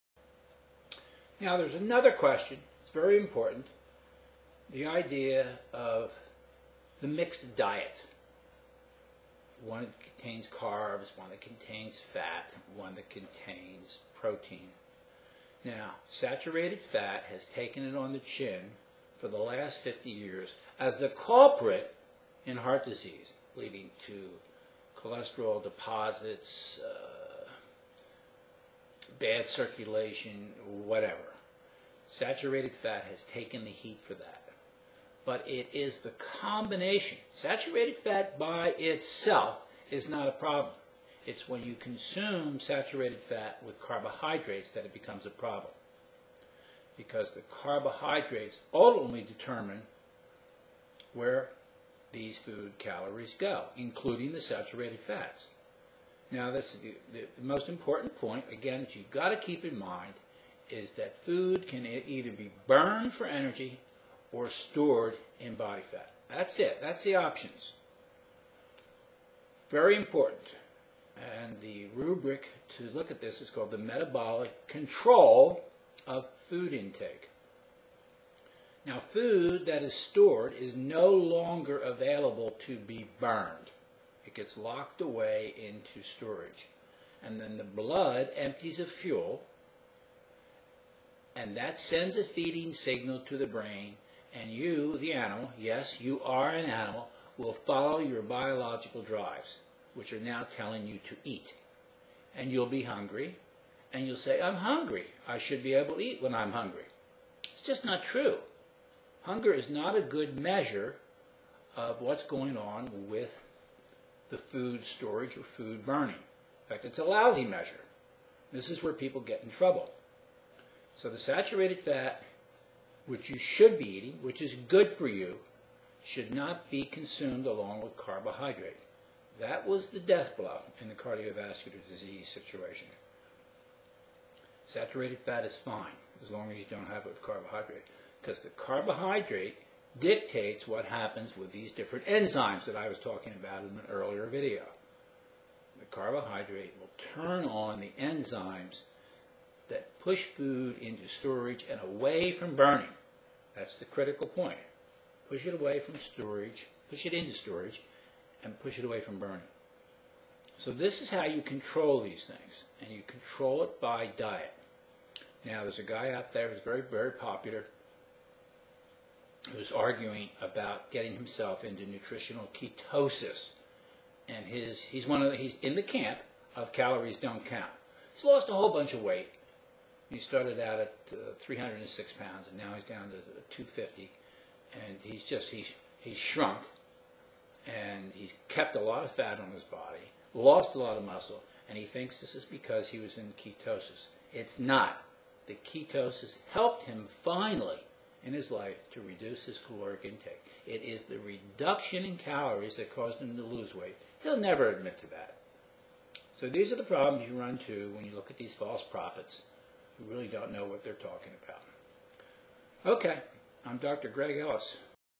Carbohydrates and Saturated Fat
Eating saturated fat and carbohydrates together creates problems and it is the carbohydates that make the fat bad because they create an environment in which both the fat and the carbohydrates get stored as fat.